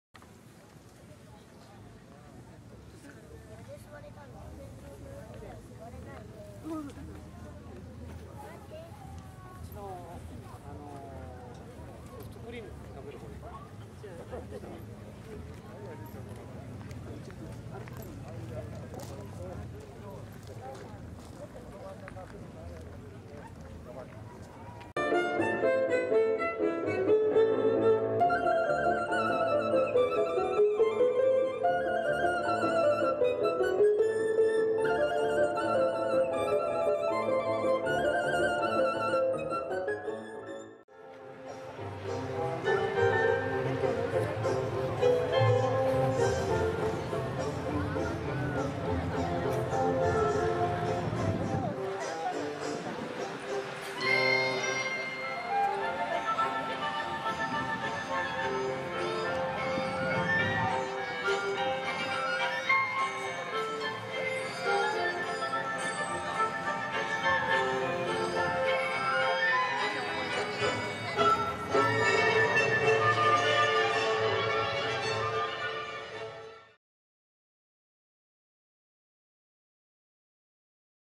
'15 八ヶ岳ポールラッシュ祭
10月の秋晴れに開催された恒例のカンティフェアーのハイライトです。